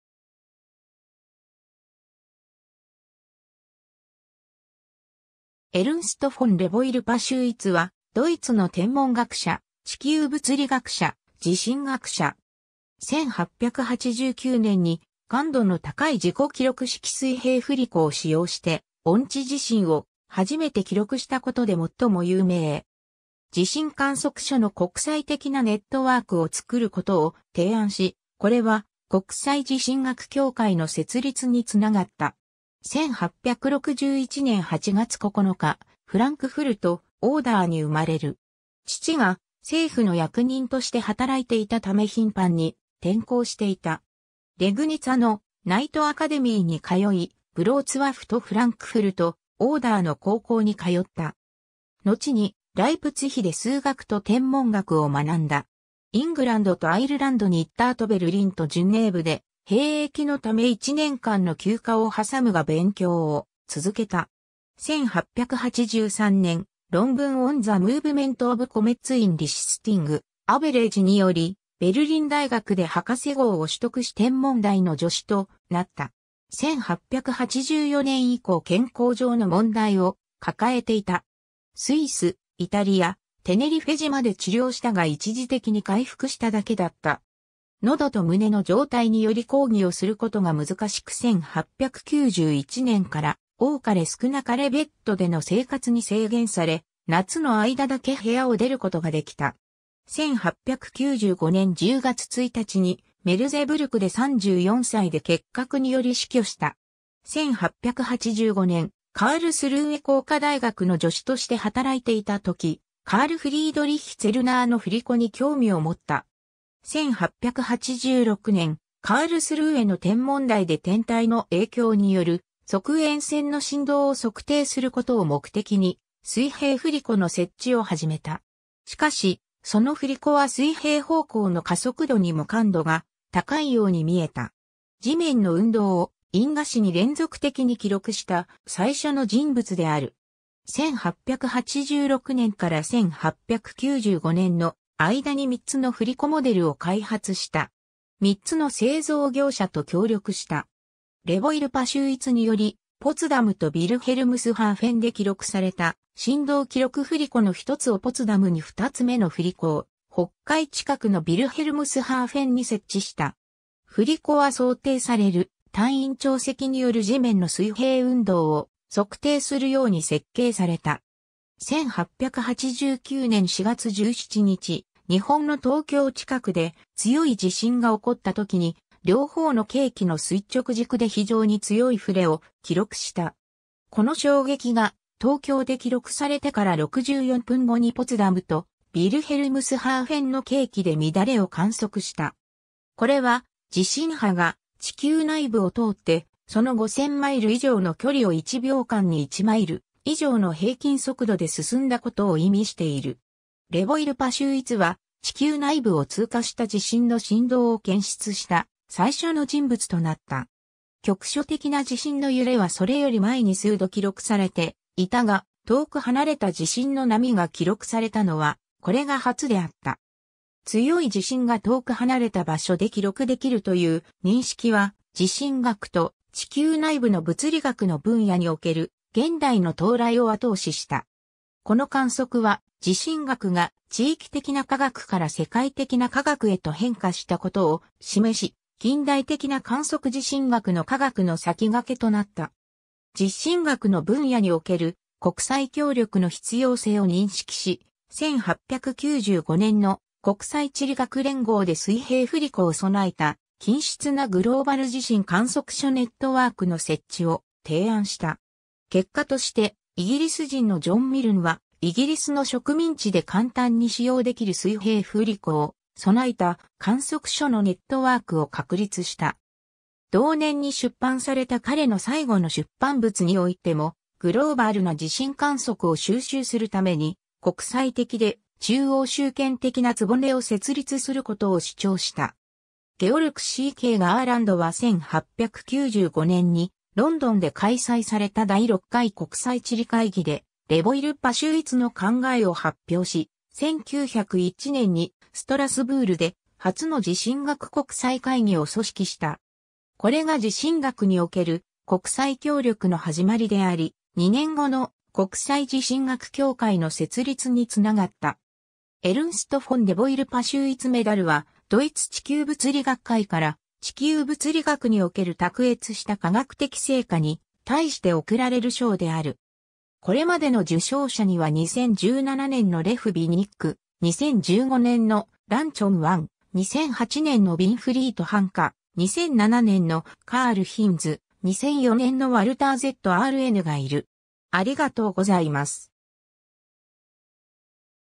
エルンスト・フォン・レボイル・パシュウィツ
エルンスト・フォン・レボイル・パシュウィツ（Ernst von Rebeur-Paschwitz、1861年8月9日、フランクフルト (オーダー) - 1895年10月1日、Merseburg）はドイツの天文学者、地球物理学者、地震学者。
1889年に感度の高い自己記録式水平振り子を使用して遠地地震を初めて記録したことで最も有名。
地震観測所の国際的なネットワークを作ることを提案し、これは国際地震学協会の設立につながった。
 1861年8月9日、フランクフルト/オーダーに生まれる。
父が政府の役人として働いていたため頻繁に転校していた。
レグニツァのナイトアカデミーに通い、ヴロツワフとフランクフルト/オーダーの高校に通った。
のちにライプツィヒで数学と天文学を学んだ。
イングランドとアイルランドに行った後ベルリンとジュネーヴで兵役のため1年間の休暇をはさむが勉強を続けた。
1883年、論文"On the Movement of Comets in Resisting Average"によりベルリン大学で博士号を取得し天文台の助手となった。
 1884年以降健康上の問題を抱えていた。
スイス、イタリア、テネリフェ島で治療したが一時的に回復しただけだった。
喉と胸の状態により講義をすることが難しく1891年から多かれ少なかれベッドでの生活に制限され、夏の間だけ部屋を出ることができた。
 1895年10月1日にメルゼブルクで34歳で結核により死去した。
 1885年、カールスルーエ工科大学の助手として働いていたとき、カール・フリードリッヒ・ツェルナーの振り子に興味を持った。
1886年、カールスルーエの天文台で天体の影響による測鉛線の振動を測定することを目的に水平振り子の設置を始めた。
しかし、その振り子は水平方向の加速度にも感度が高いように見えた。
 地面の運動を印画紙に連続的に記録した最初の人物である。
1886年から1895年の間に3つの振り子モデルを開発した。
3つの製造業者と協力した（1886年にFecker振り子、1888年にRepsold振り子、1894年にStückrath two-component model）。
 レボイル・パシュウィツによりポツダムとヴィルヘルムスハーフェンで記録された震動記録 振り子の1つをポツダムに2つ目の振り子を北海近くのヴィルヘルムスハーフェンに設置した。
振り子は想定される太陰潮汐による地面の水平運動を測定するように設計された。
1889年4月17日、日本の東京近くで強い地震が起こったときに両方の計器の垂直軸で非常に強いふれを記録した。
この衝撃が東京で記録されてから64分後にポツダムとヴィルヘルムスハーフェンの計器で乱れを観測した。
これは地震波が地球内部を通ってその5000マイル以上の距離を1秒間に1マイル以上の平均速度で進んだことを意味している。
レボイル・パシュウィツは地球内部を通過した地震の震動を検出した最初の人物となった。
局所的な地震の揺れはそれより前に数度記録されていたが、遠く離れた地震の波が記録されたのはこれが初であった。
強い地震が遠く離れた場所で記録できるという認識は地震学と地球内部の物理学の分野における現代の到来を後押しした。
 この観測は地震学が地域的な科学から世界的な科学へと変化したことを示し、近代的な観測地震学の科学の先駆けとなった。
 地震学の分野における国際協力の必要性を認識し、1895年の国際地理学連合で水平振り子を備えた均質なグローバル地震観測所ネットワークの設置を提案した。
結果としてイギリス人のジョン・ミルンはイギリスの植民地で簡単に使用できる水平振り子を備えた観測所のネットワークを確立した。
 同年に出版された彼の最後の出版物においてもグローバルな地震観測を収集するために国際的で中央集権的な局を設立することを主張した。
 Georg C. K. Gerlandは1895年にロンドンで開催された第6回国際地理会議でレボイル・パシュウィツの考えを発表し、1901年にストラスブールで初の地震学国際会議を組織した。
これが地震学における国際協力の始まりであり、2年後の国際地震学協会（1951年より国際地震学及び地球内部物理学協会(IASPEI)）の設立につながった。
 エルンスト・フォン・レボイル・パシ...